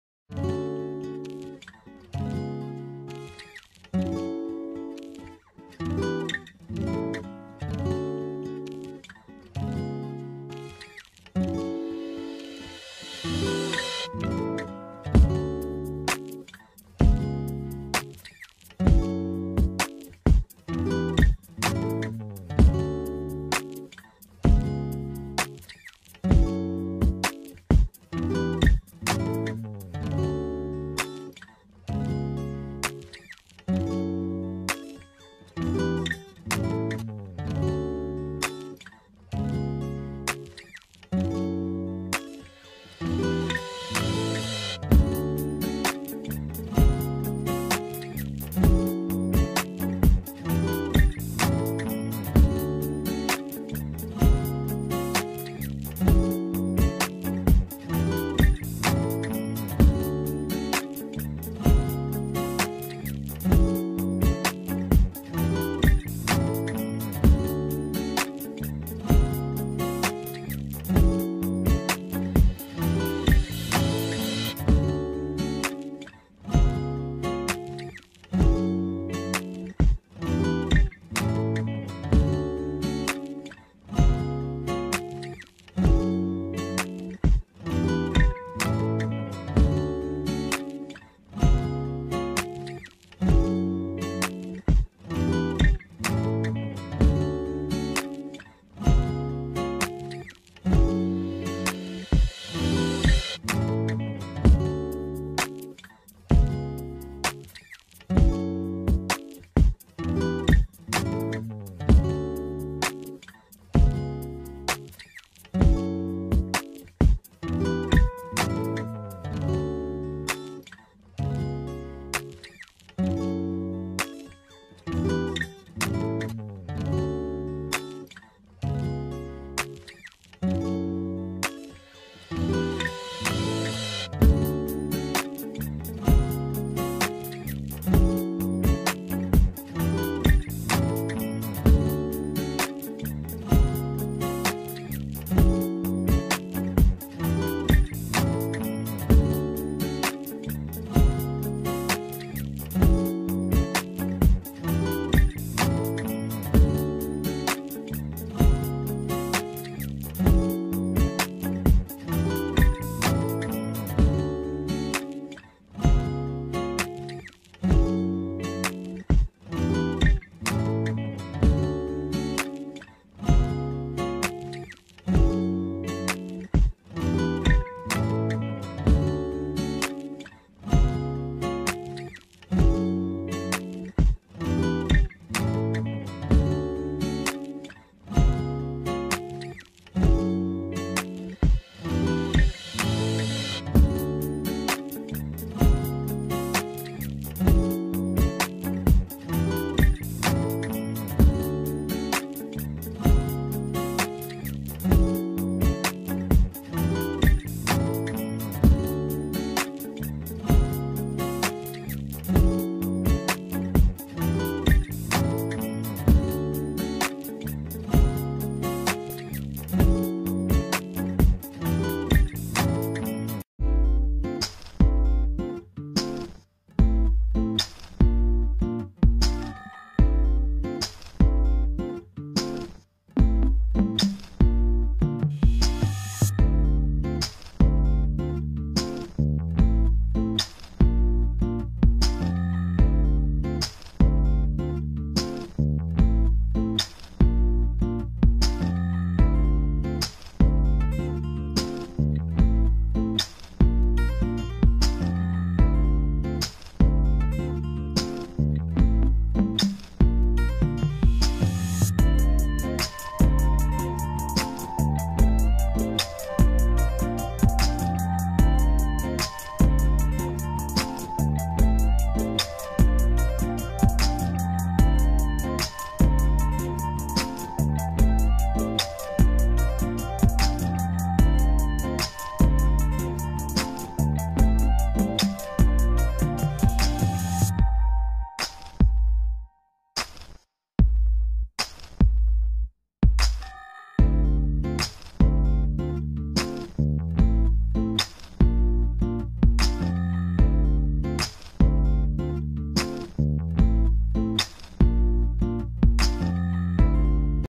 Get ready with me, natural fresh sweet look
Products I'm using in this video

Hard Candy glamoflage in Very Light (but the link is for the light one)

Soho foundation brush
(can't find a link to it)

Rimmel London scandal eyes waterproof khol kajal in nude

LA Colors lash building mascara (mini)